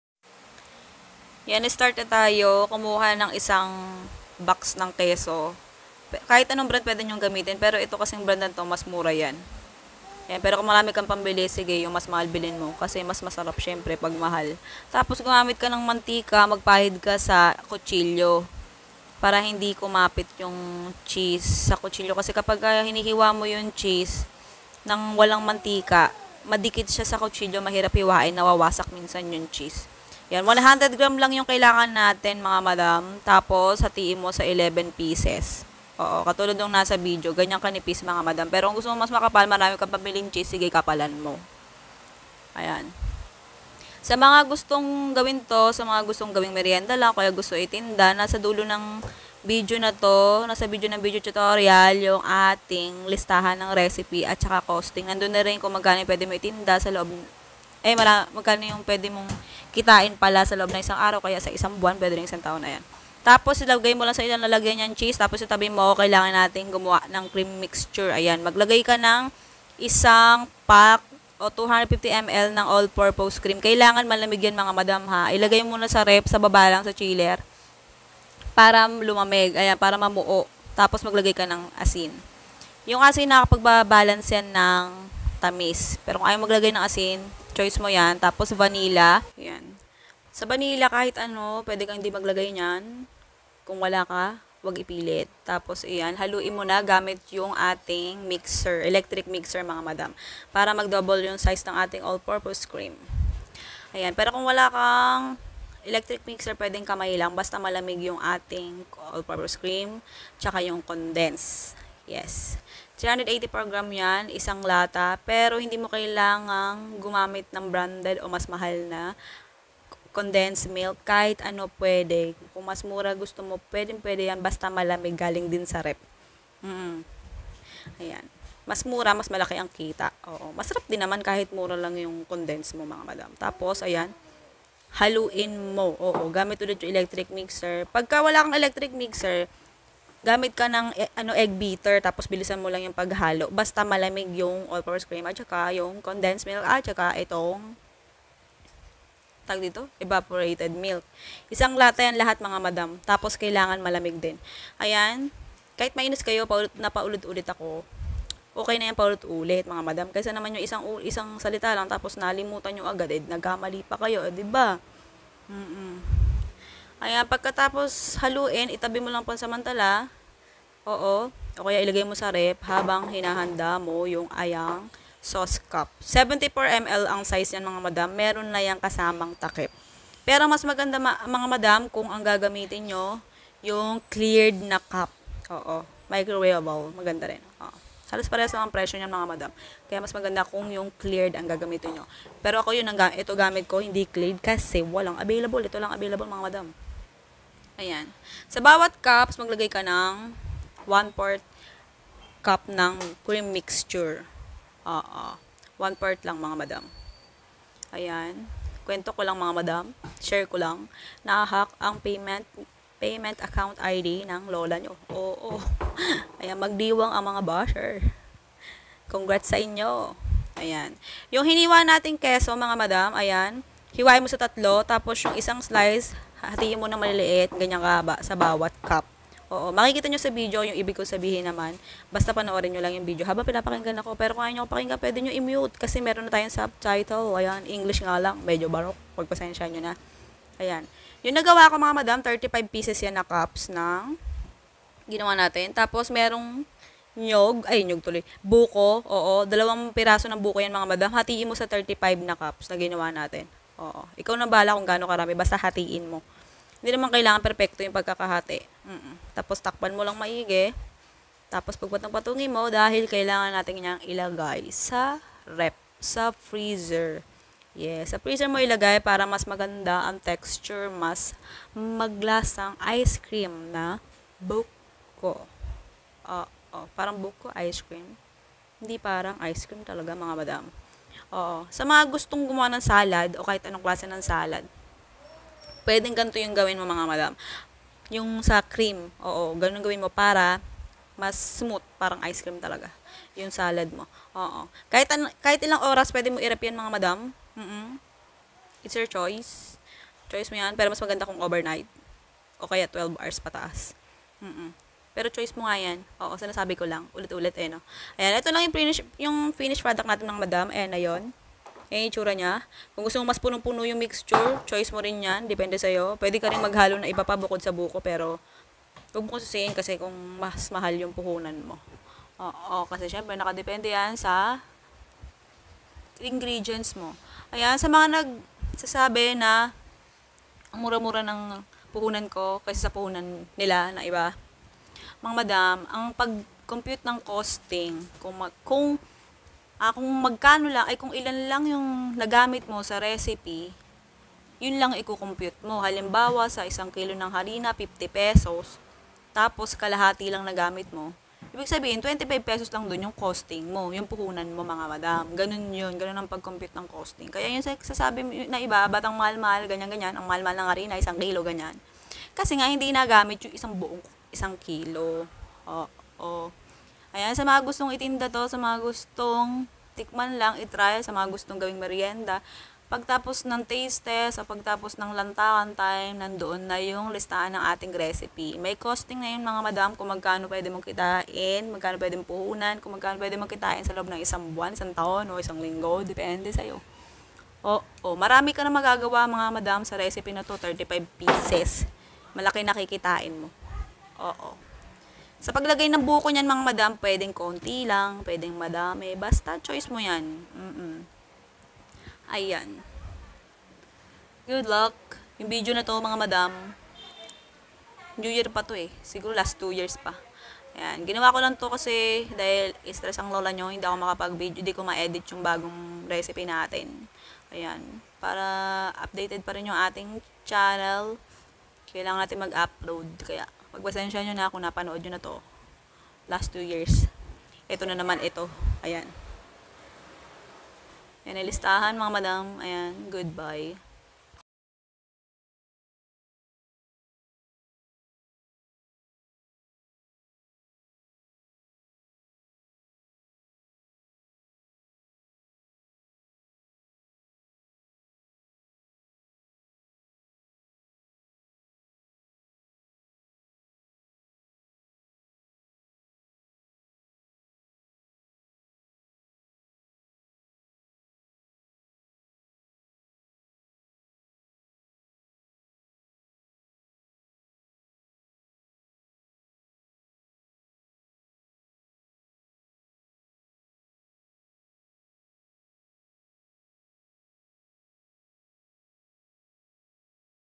NEGOSYONG MALAKI ANG KITA TIYAK NA PATOK NGAYUNG 2022 AT SA MGA SUSUNOD PA | Halomix Foodypar
INGREDIENTS WITH COSTING

2pc. Buko = P100
1 big 384g. can Evaporated milk = P32
1pouch 250g. All purpose cream = P54.50
1 big 370g.  can condensed milk = P57
100g. Cheese = P22
1/2tsp vanilla = P.25
1/4tsp iodized salt = P.25
35pcs. 74ml. Sauce cups = P45.50

COSTING

YIELD= 35cups
PUHUNAN= P311.50
BENTA KADA ISANG CUP= P15 each
KABUUANG BENTA= P525
TUBO= P213.50

TUBO PER MONTH= P213.50 ×30 days = P6,405

TUBO PER YEAR= P6,405 ×12months.= P76,860